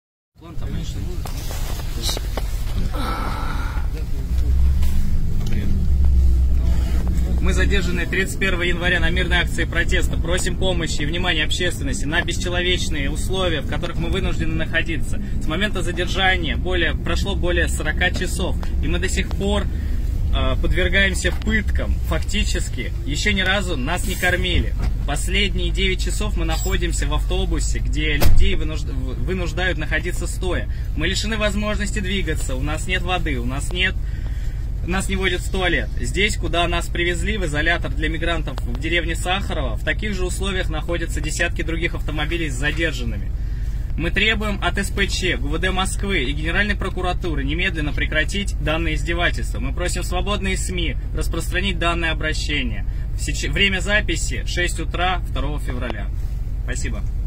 Задержанные 31 января находятся в автозаках без воды и еды
Они записали обращение с требованием прекратить пытки